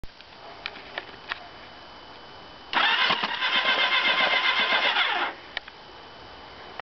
Hayabusa - wytrysk
Przy sprawdzaniu szczelności zaworów ssących.